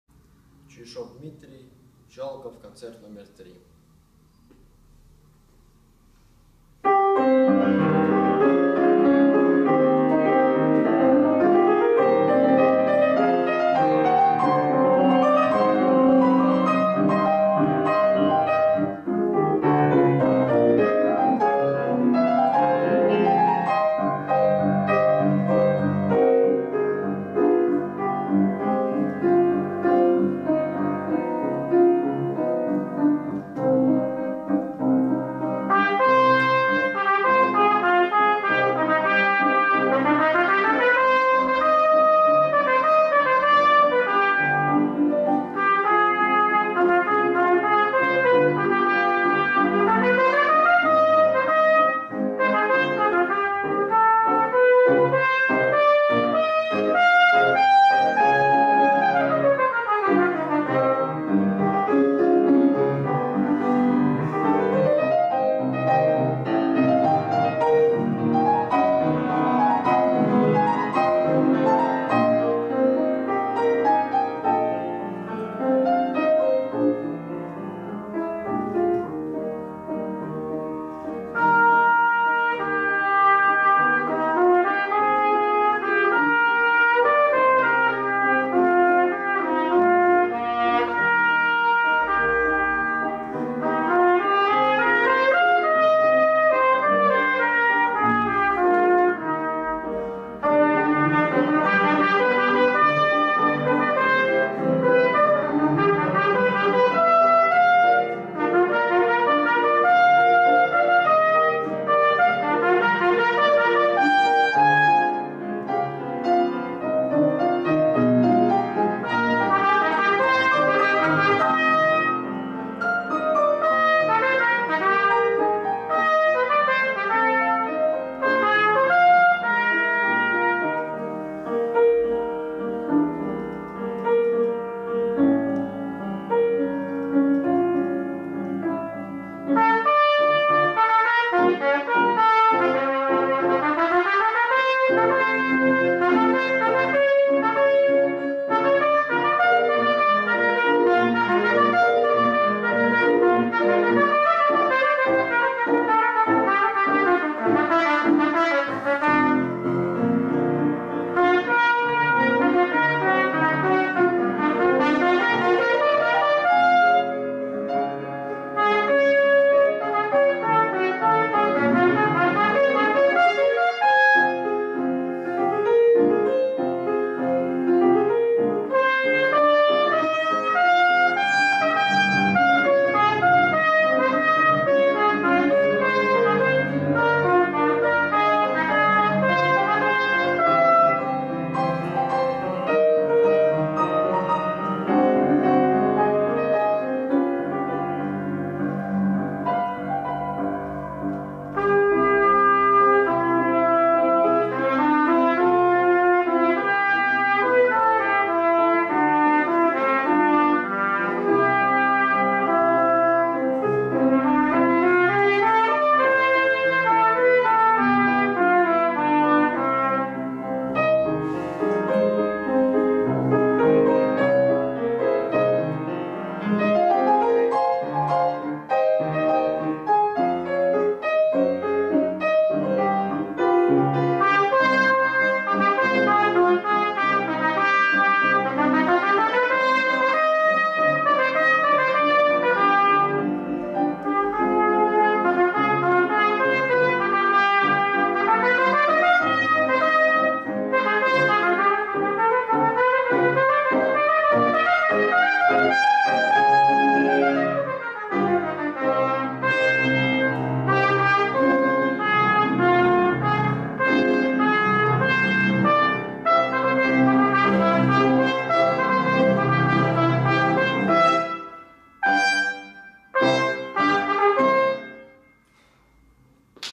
CS-XV-0013(2)Dmitry Chuyashov,Instrumental music, Wind instr., Solo, Elder group, RF, St.Petersburg
CS-XV-0013 (2) Dmitry Chuyashov, Instrumental music, Wind instruments, Solo, Elder group, RUSSIAN FEDERATION, Saint-Petersburg